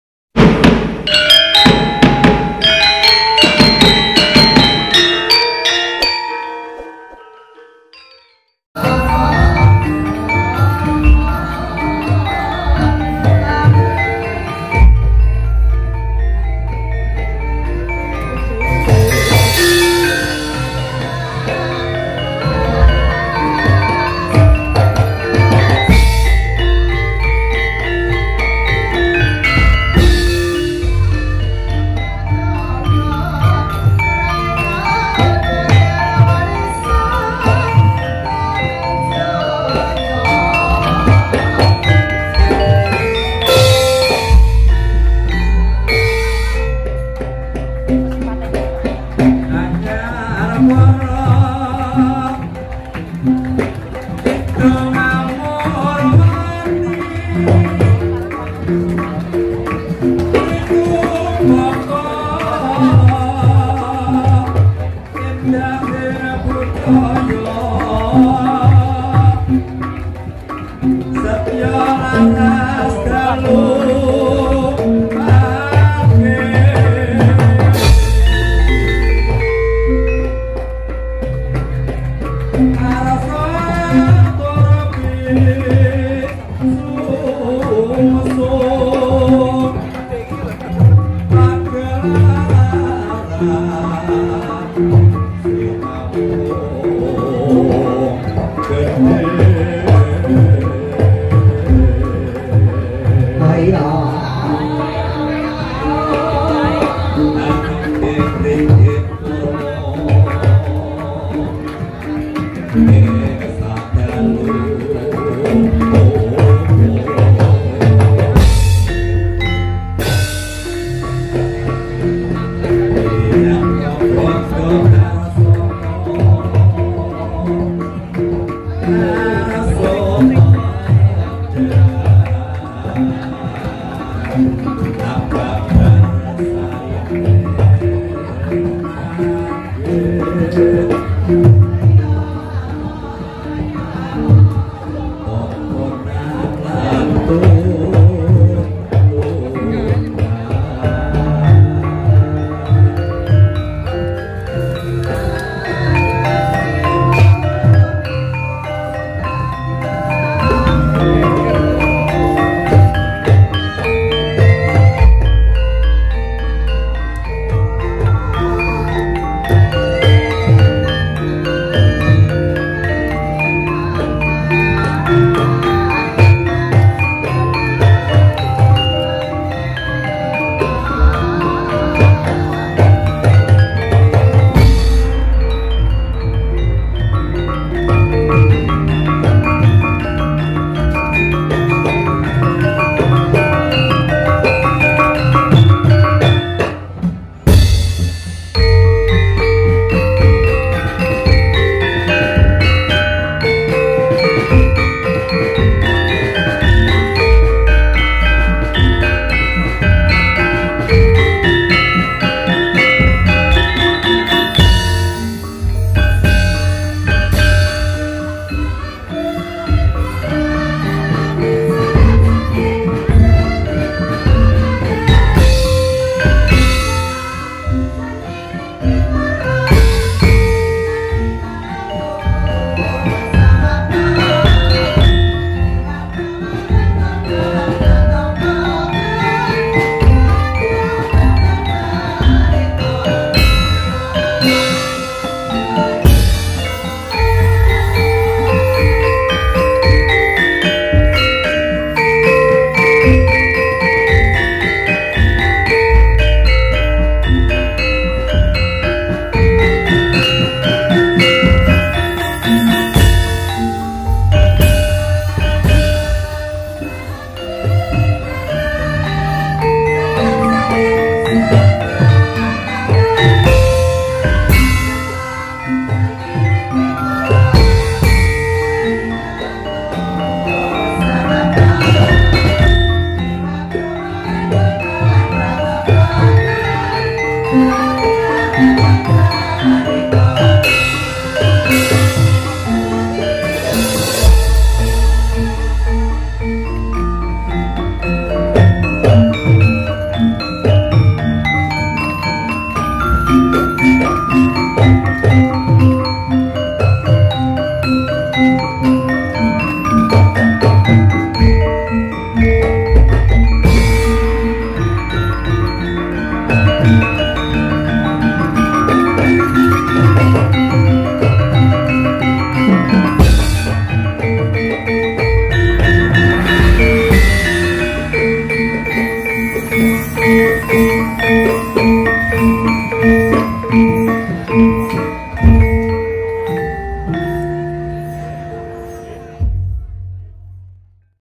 Pra Pentas Wayang Kulit Ki Yatno Gondo Darsono ! Morosari Sukorejo Ponorogo
Video tentang persiapan da tes gamelan maupun para pengrawit Dalang Ki Yatno Gondo Darsono Morosari Sukorjo Pomorogo.